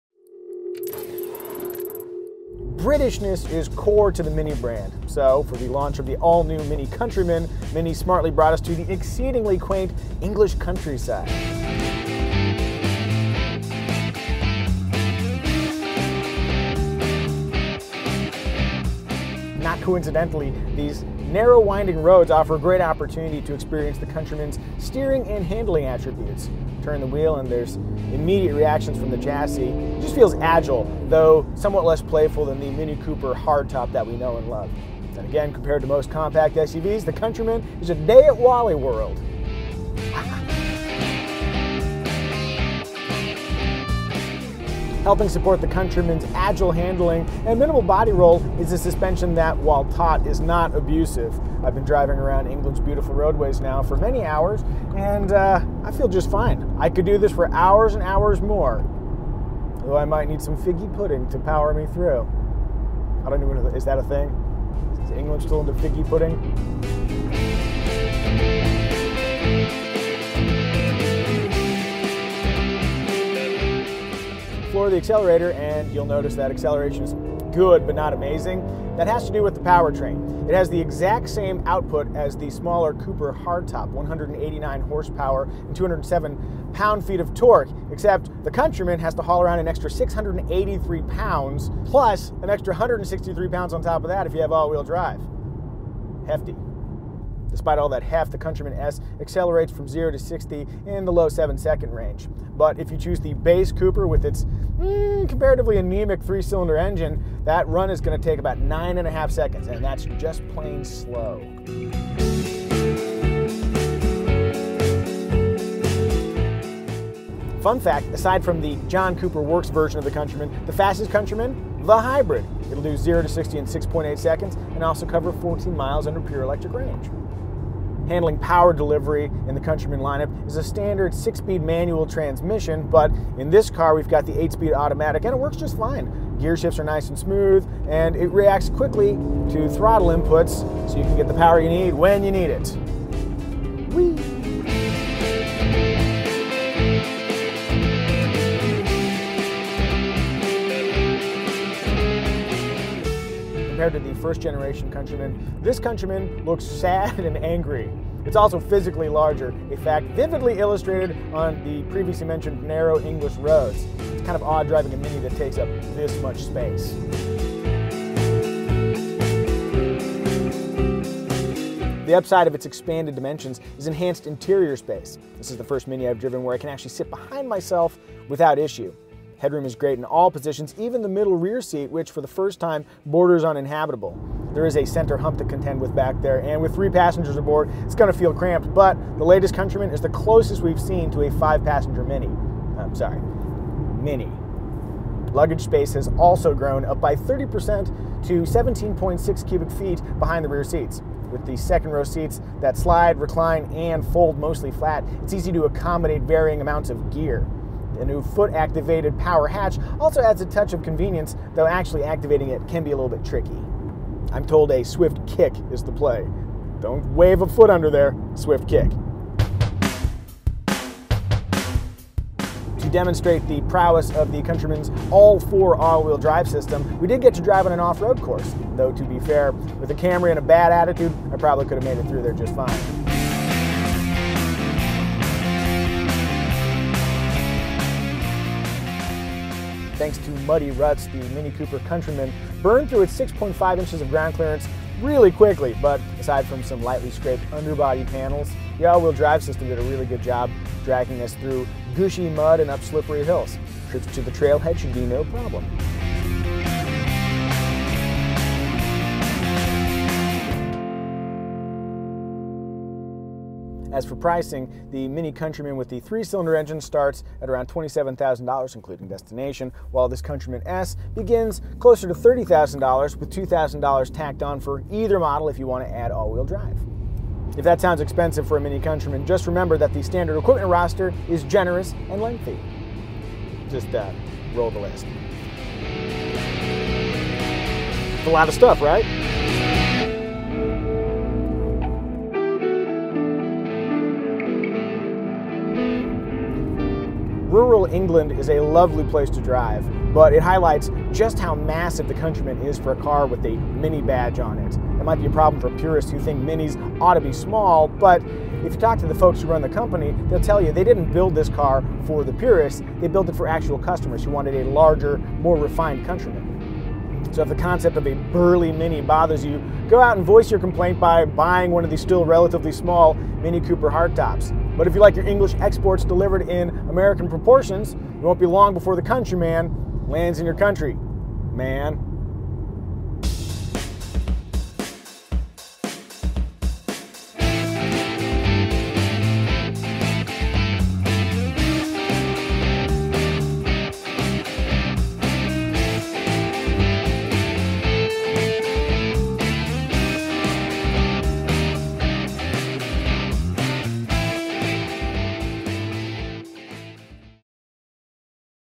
2017 Mini Countryman - First Look
KBB's First Look at the 2017 Mini Countryman.

Kelley Blue Book is your source for new car reviews, auto show coverage, features, and comparison tests.